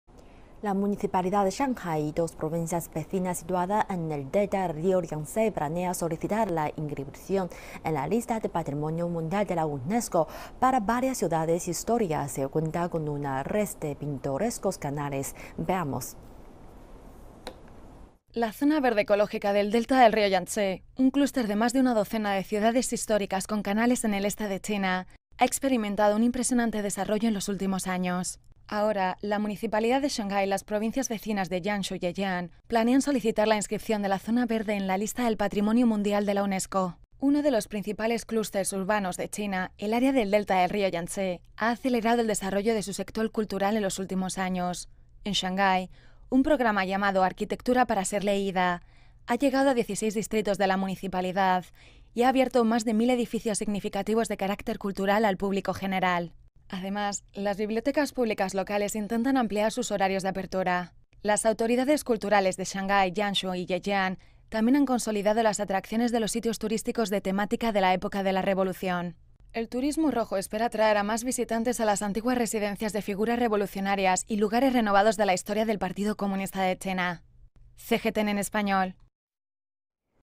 Shanghai y dos provincias vecinas solicitarán inscripción en la lista del Patrimonio Mundial
La municipalidad de Shanghai y dos provincias vecinas situada en el Delta del Río Yangtzé, planean solicitar la inscripción en la lista del Patrimonio Mundial de la UNESCO para varias ciudades históricas que cuentan con una red de pintorescos canales.